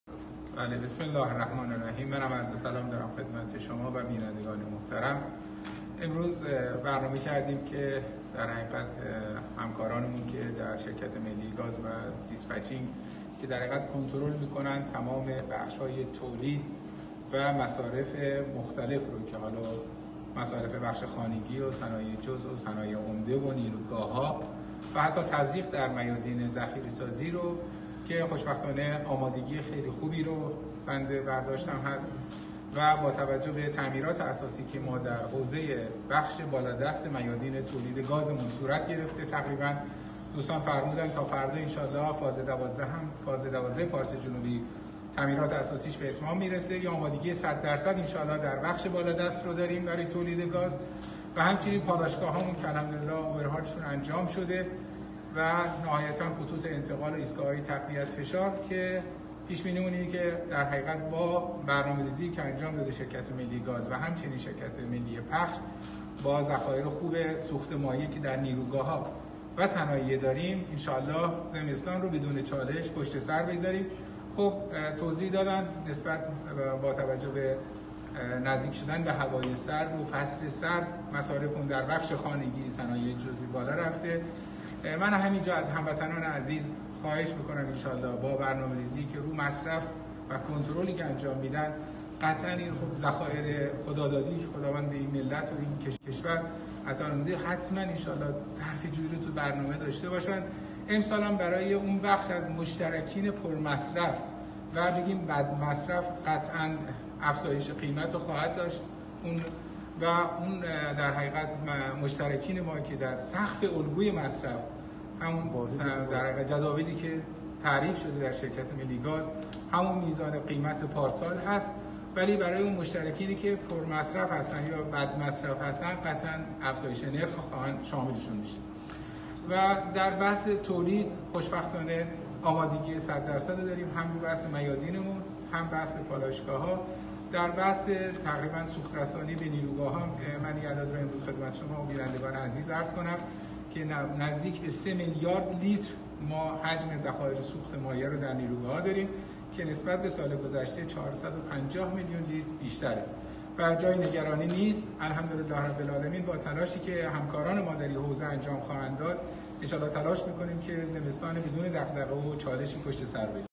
اوجی، وزیر نفت پس از بازدید سرزده روز جمعه (هفتم آبان‌ماه) از مدیریت دیسپچینگ شرکت ملی گاز ایران:
در بخش تولید گاز آمادگی ۱۰۰ درصدی داریم و پیش‌بینی این است که با برنامه‌ریزی  شرکت‌های ملی گاز و  پخش فرآورده‌های نفتی و در عین حال ذخیره‌سازی سوخت مایع مناسب در نیروگاه‌ها و صنایع ان‌شالله زمستان امسال را بدون چالش پشت می‌گذاریم.